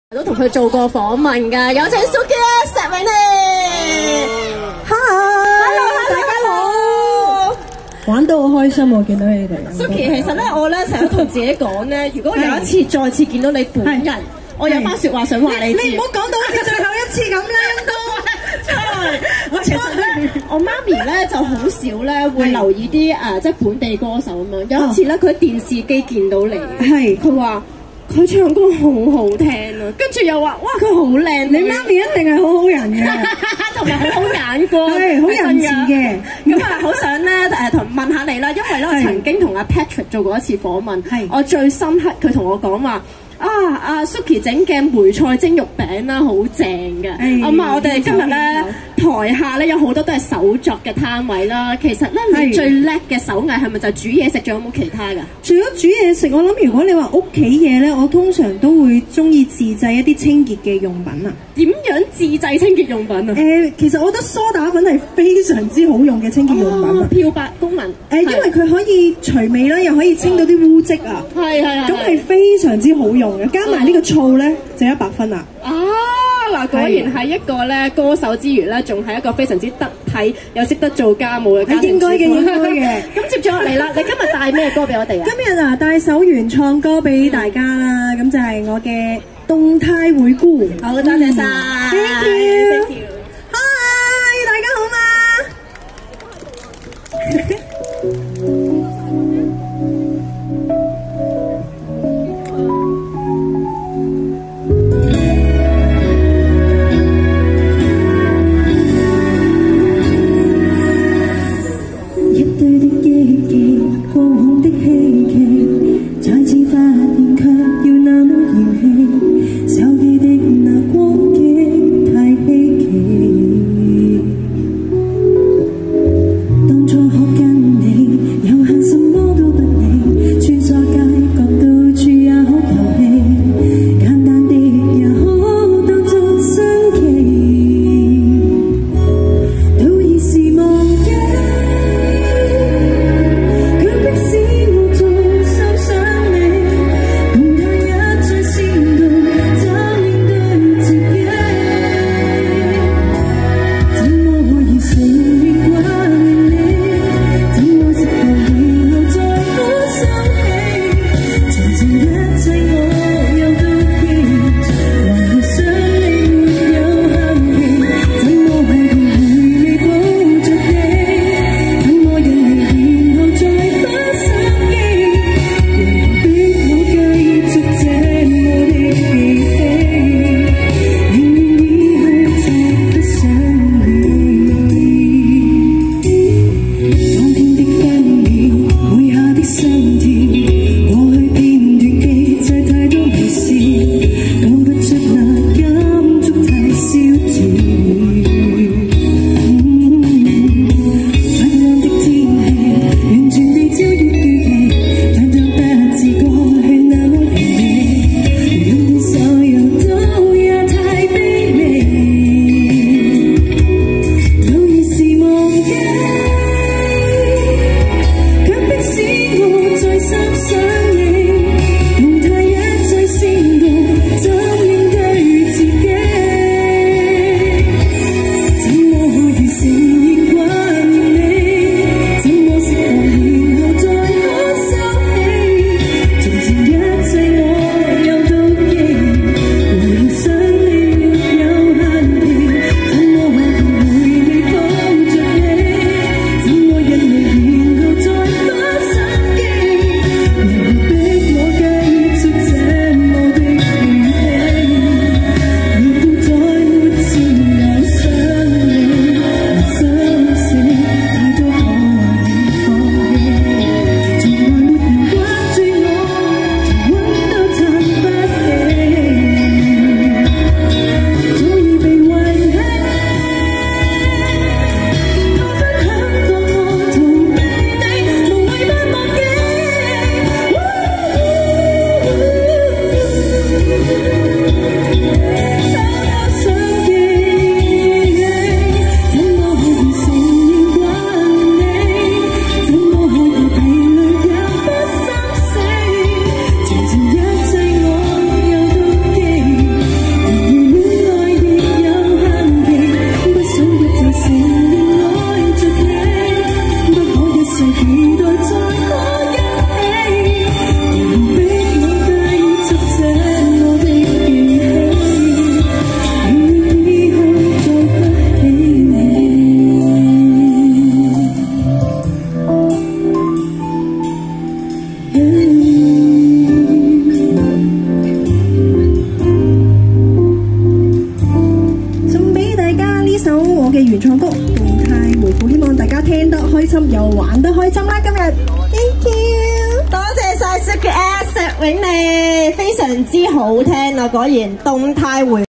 石詠莉-動態回顧(2019年12月15日長沙灣遊樂場)
司儀向美玲,super girls 兩個去二次採訪,由於早前已影左相,估量下認為拍下完整唱歌片更重要